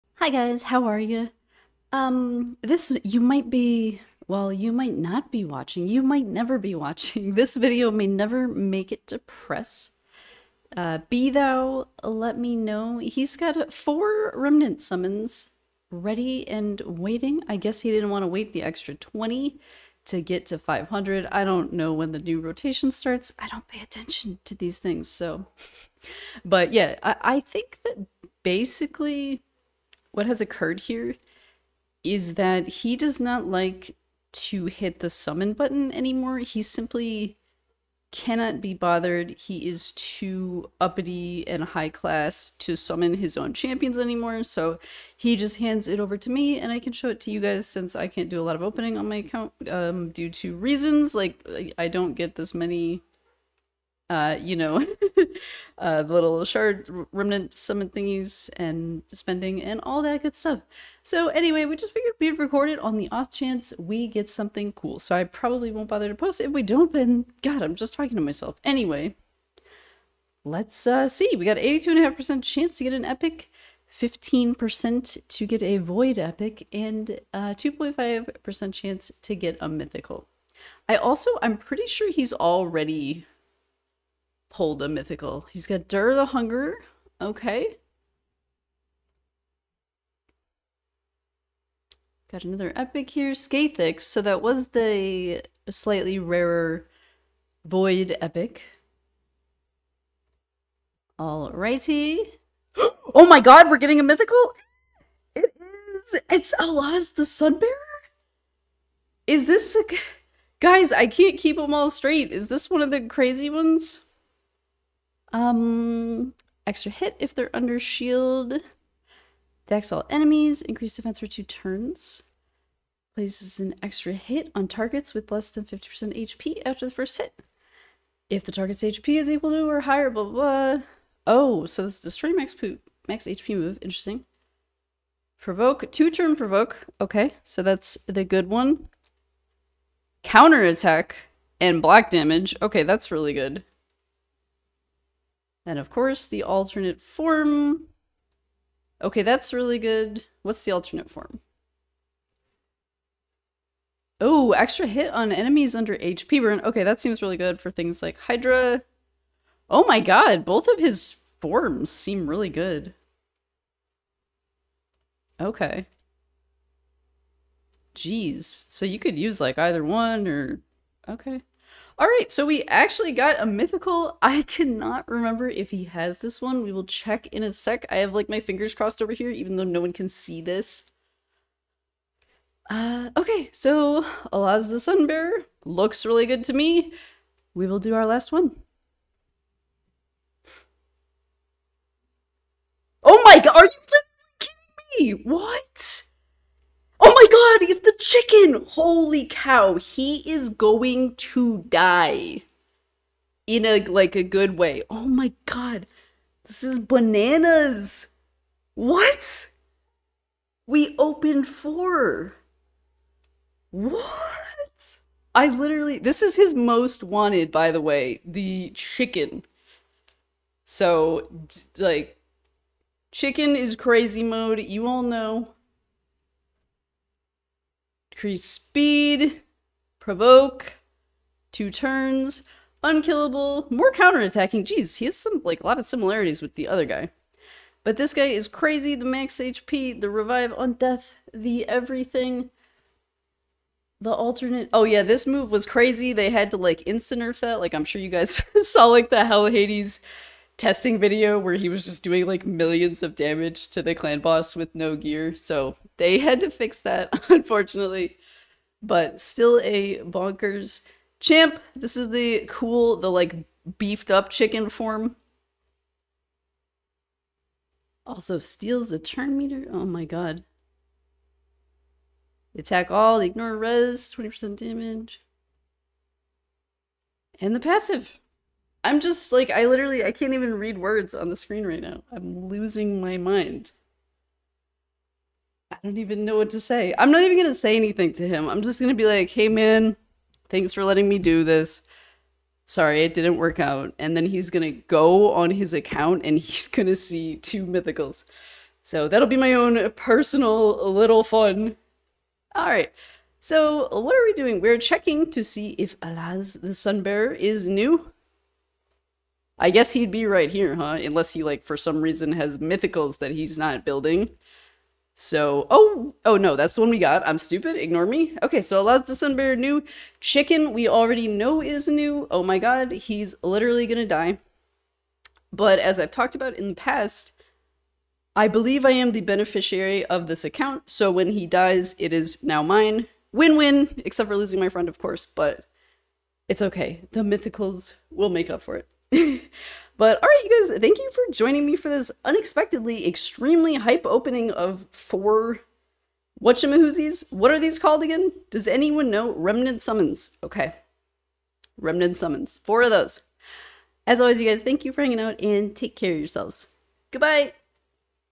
MOST INSANE REMNANT SUMMONS EVER?!
Be Thou asked me to open his remnant summons just to see what would happen... and... well... this happened! 👀